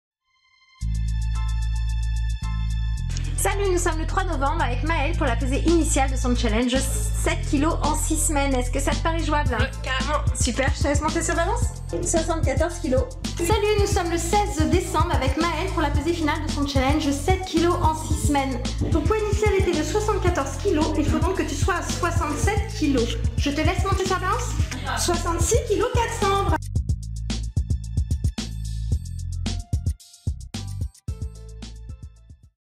Transformation physique rapide Nantes
Maelle a perdu 7,600 kgs en 6 semaines à la Training Academy